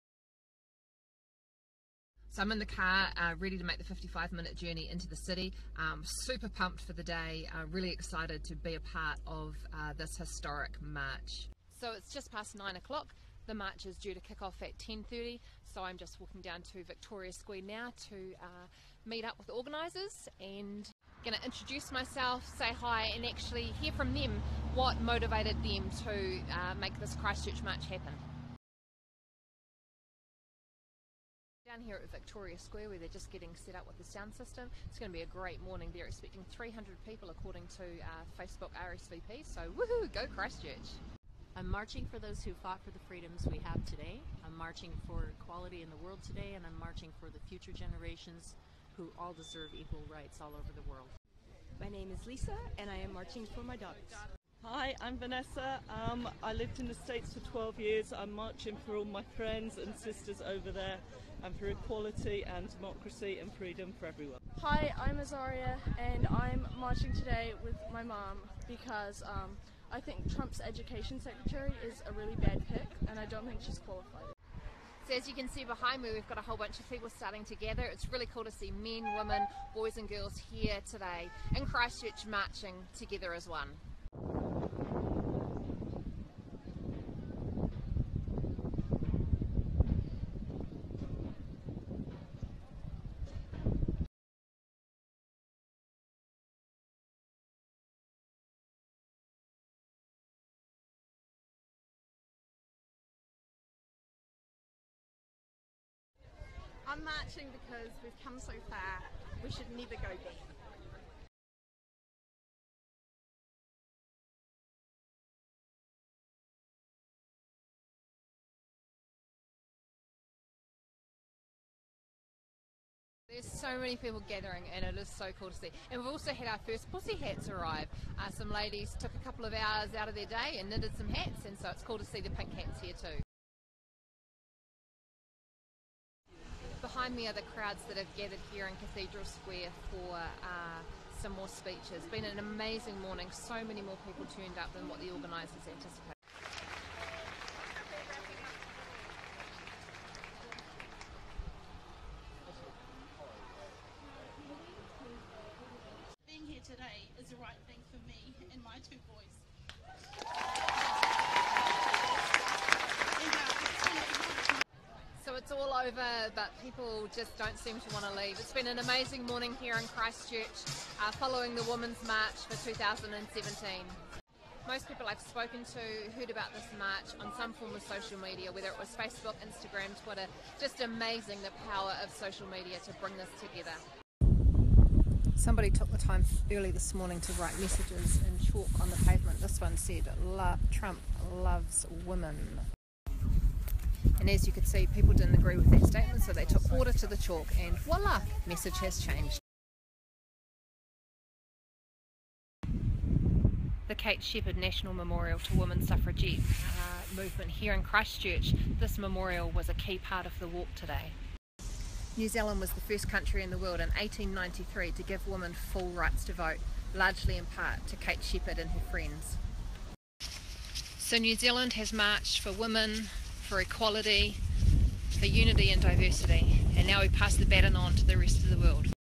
Women's March via Shar's Snapchat
I went to the Women's March in Christchurch